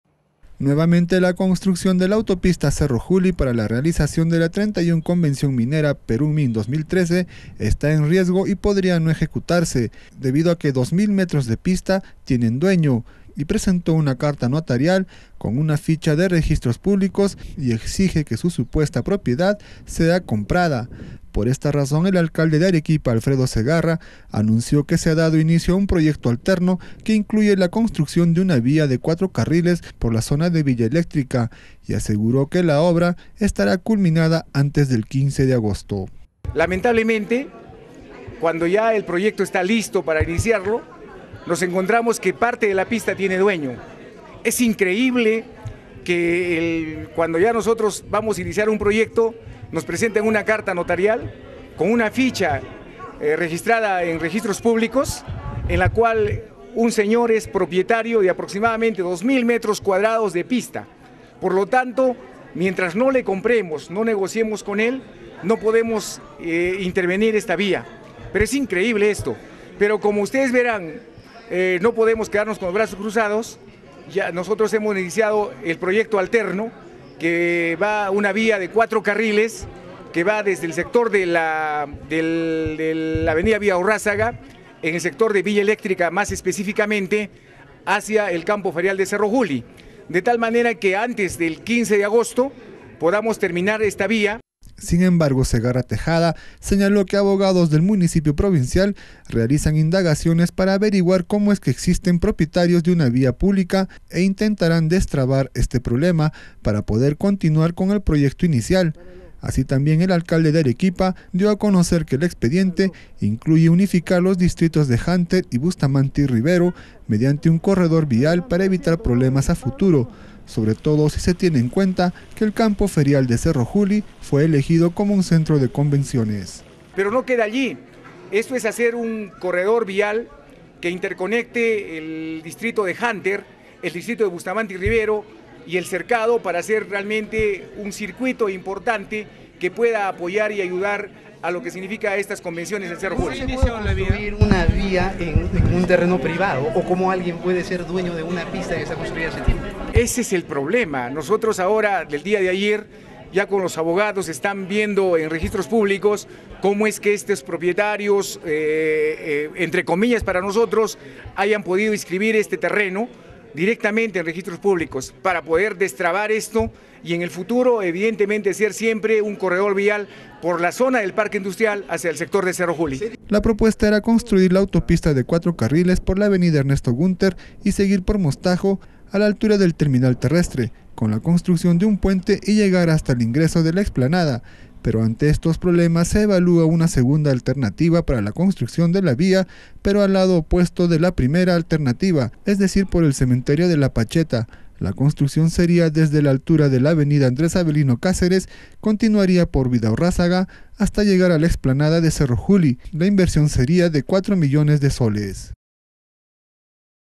Nuevamente corre riesgo la construcción de la autopista al campo ferial de Cerro Juli
Aparecieron supuestos dueños de 2 mil metros de pista, por ello el Alcalde Alfredo Zegarra anunció el inicio de un proyecto alterno.